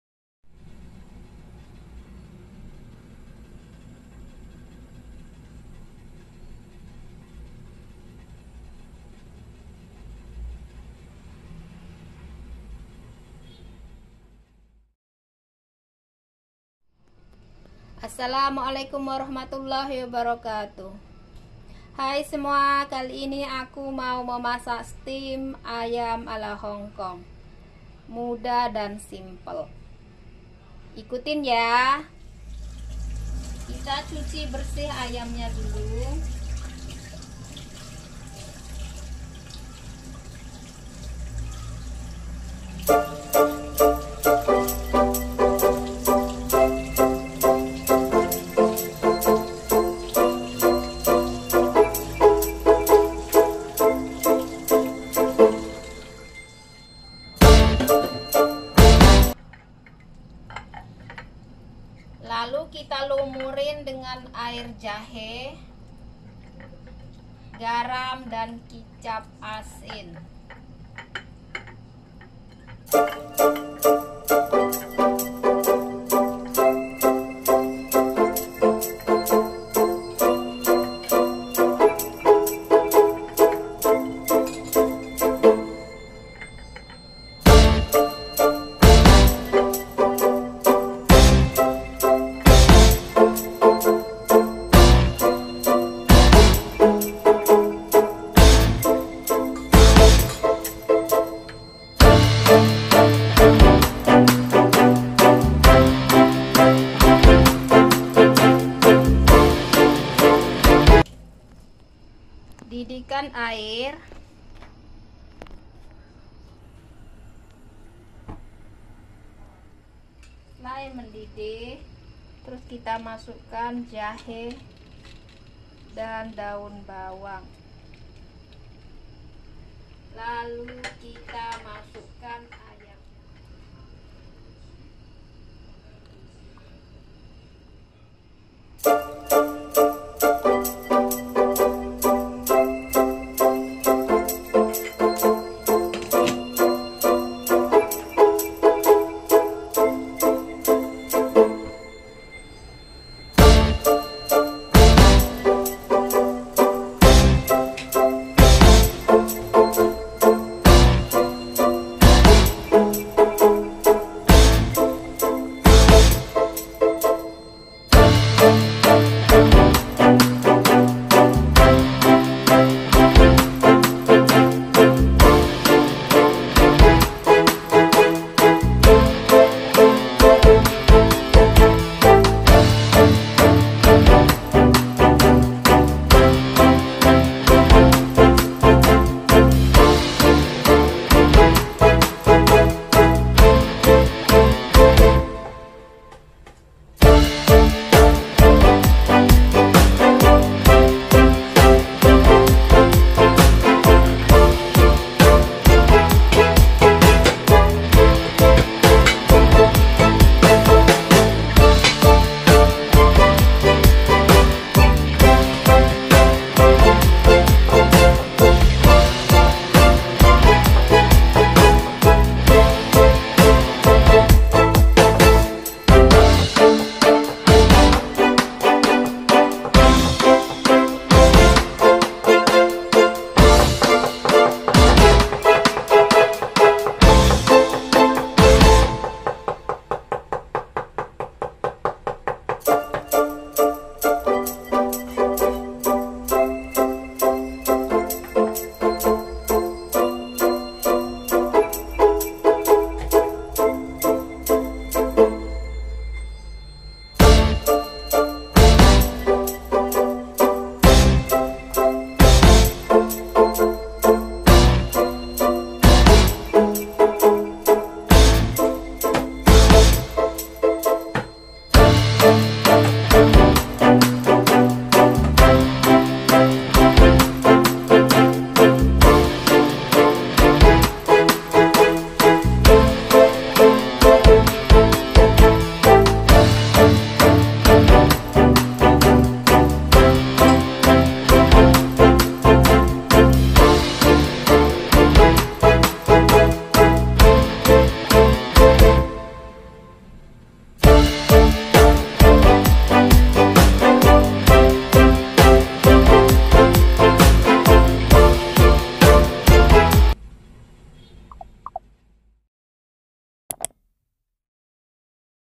Masakan sehari-hari hongkong steam ayam Pak cik kai
Cara memasak steam ayam ala hongkong masakan sehari -hari yang mudah enak dan simple
Bahannya
1 kg daging ayam
1 ruas Jahe
2 batang daun bawang
1 sdm garam
2 sdm air perasan jahe
2 Sdm kecap asin

Caranya : Tonton video ini sampai habis ya...
Terima Kasih